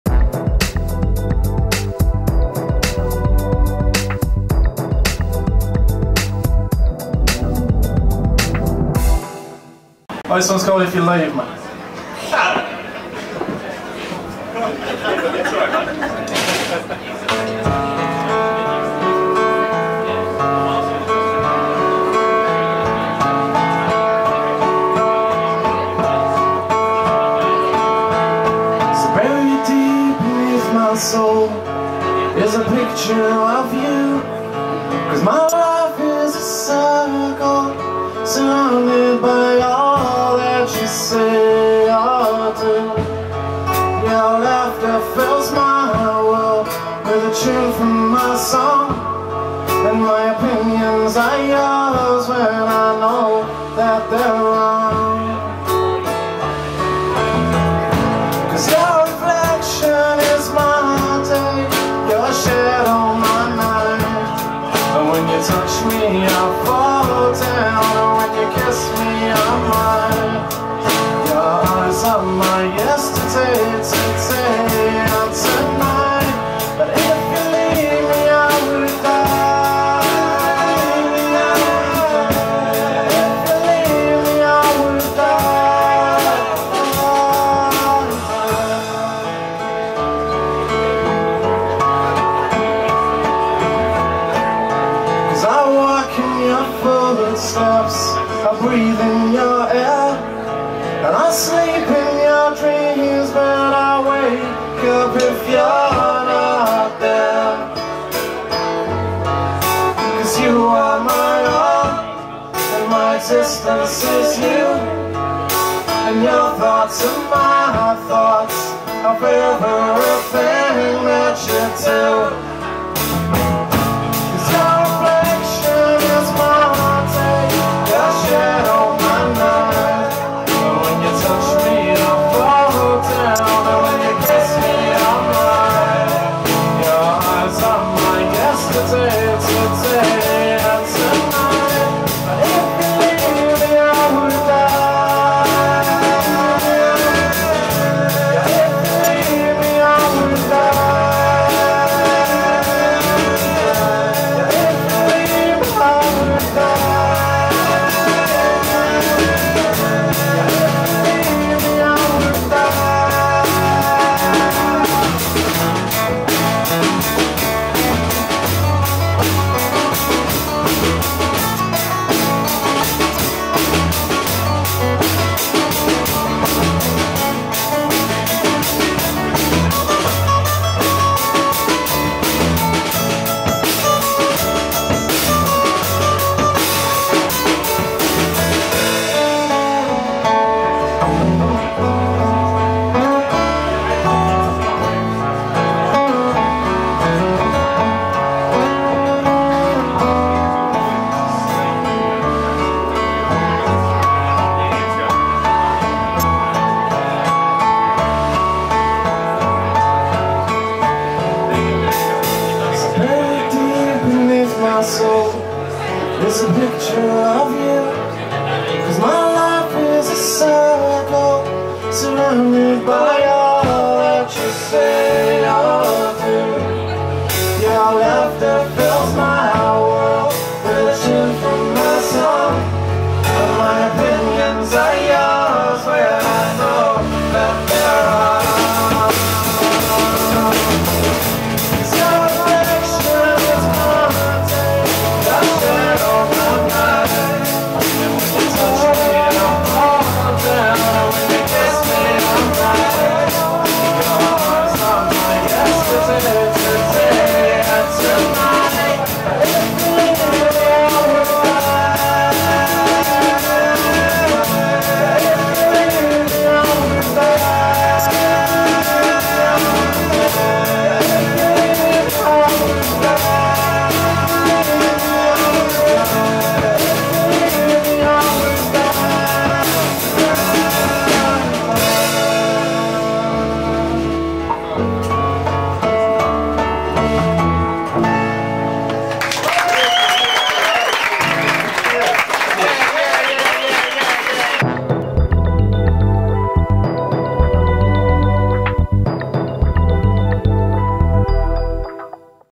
The Wallchasers Live At 12 Bar Club London For OnlineTV by Rick Siegel
The Wallchasers Live At 12 Bar Club London For OnlineTV by Rick Siegel Sep 6, 1999 for more information or to order complete concerts.

THUMBS UP if I gave you something you liked!